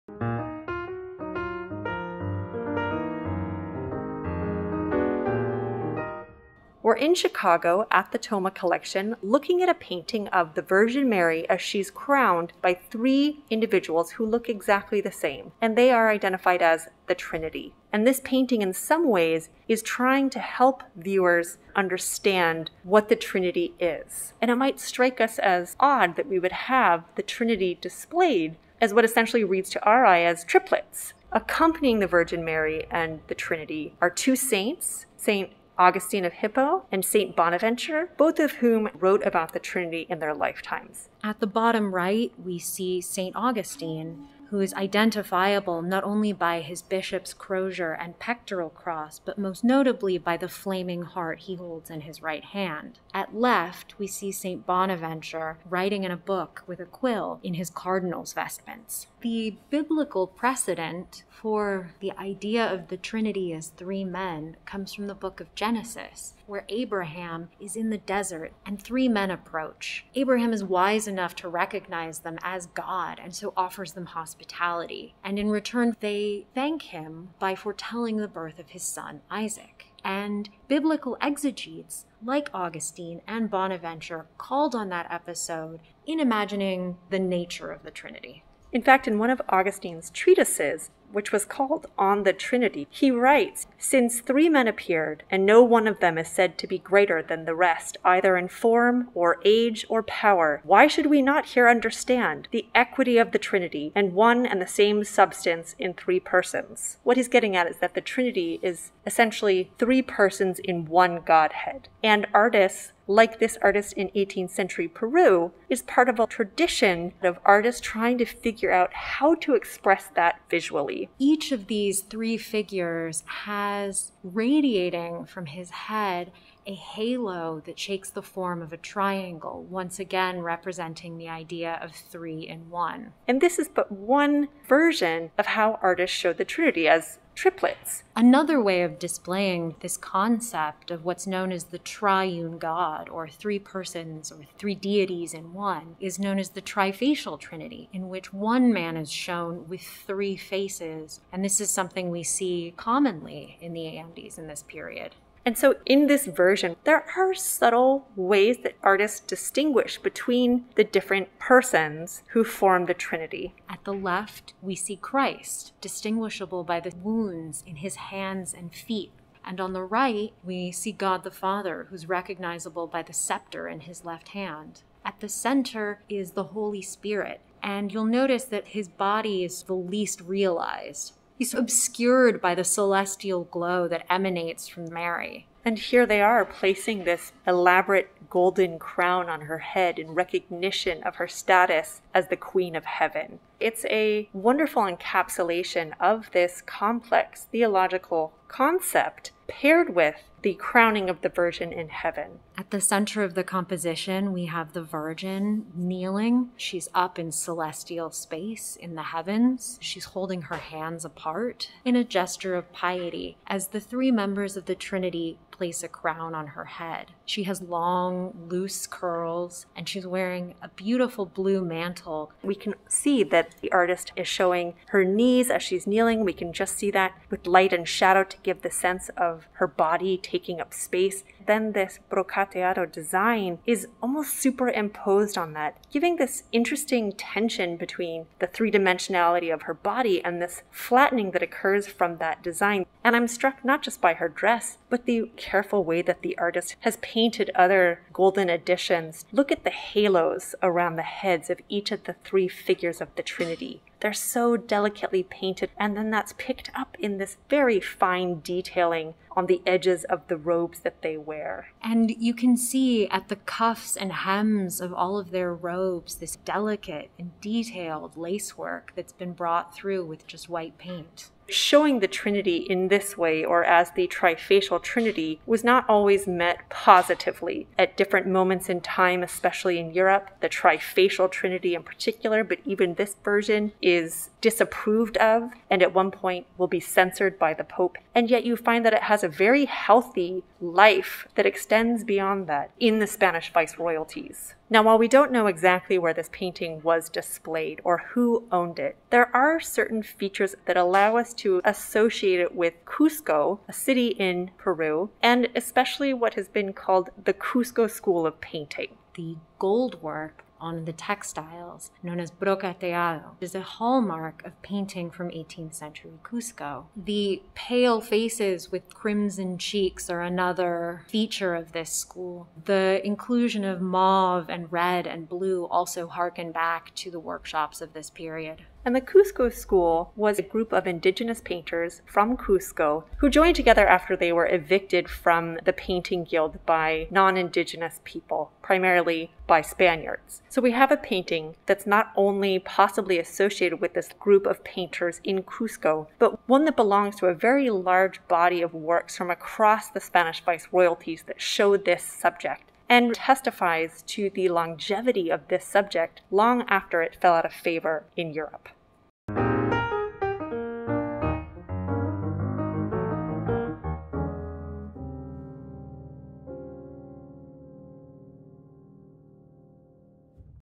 One God in three: The Coronation of the Virgin by the Holy Trinity
A conversation between Dr. Kathryn Santner and Dr. Lauren Kilroy-Ewbank in front of The Coronation of the Virgin by the Holy Trinity, 18th century, oil and gold on canvas, Cusco, Peru 78.105 x 59.055 cm (Collection of the Carl & Marilynn Thoma Foundation)